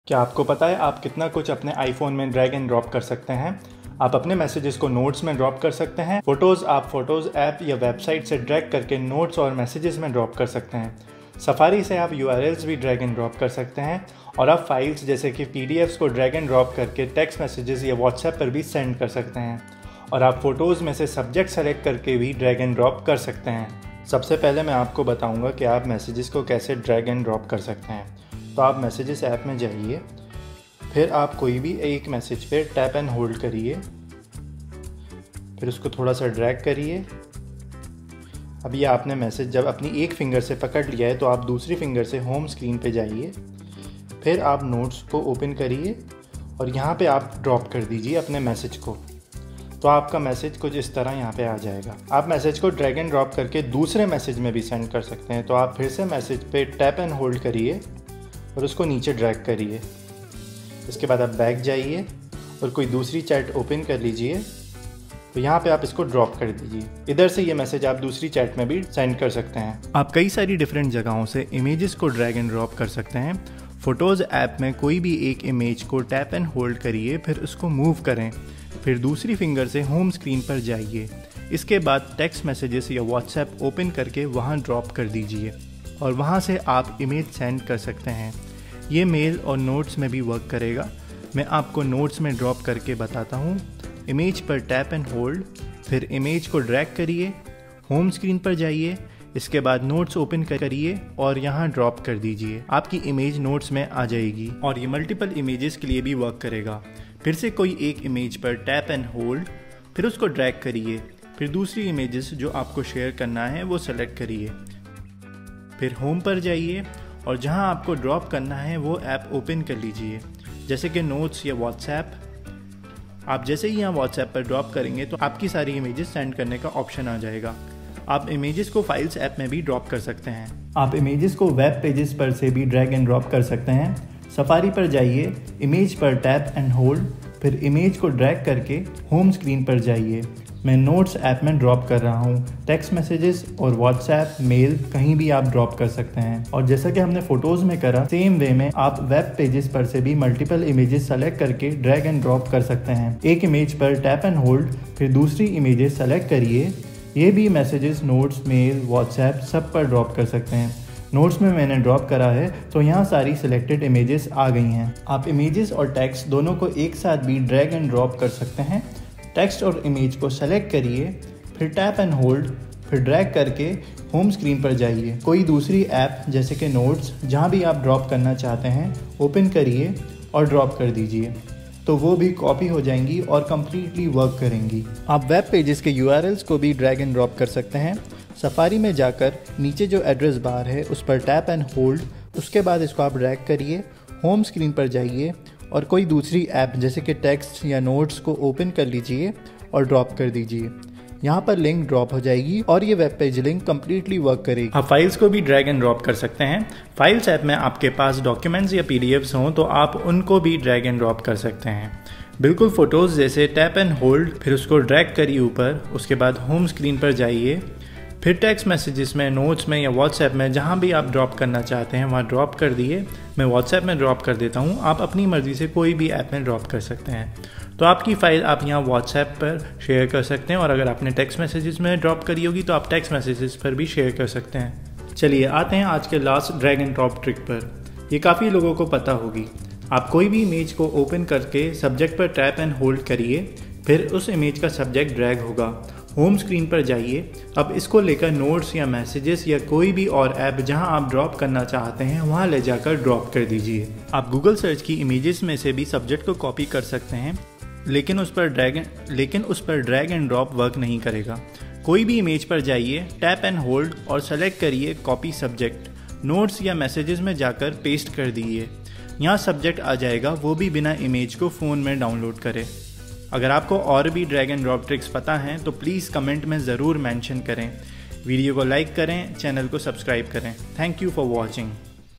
iPhone Drag & Drop Tricks | (Secret Tricks!)
In this video, I will show you some secret but easy tricks to drag & drop tricks. These trip will change your iPhone experience, many of these will also work on iPads. Hope you will learn something new.
And if you know anymore drag & drop tricks, please mention them in comments section.

Chapters
00:00 - Intro
00:23 - Tip 1 Messages
01:09 - Tip 2 Photos
02:10 - Tip 3 Web Images
02:44 - Tip 4 Texts & Images
03:05 - Tip 5 URLs
03:24 - Tip 6 Files
03:55 - Tip 7 Subjects from Images